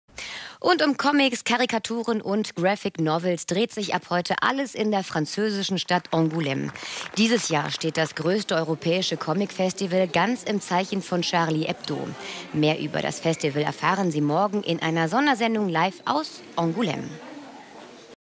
42. Intern. Comicfestival in Angoulême 2015
Der französisch-deutsche Fernsehsender Arte bringt am 30.01. eine Sondersendung zum größten Comicfestival in Europa. Diese Messe findet im französischen Angoulême vom 29.1. bis zum 1.2. statt. (Die Ankündigung hörte sich größer an, als es die "Sondersendung" am 30.1. war, aber immerhin.)
Hier zu sehen: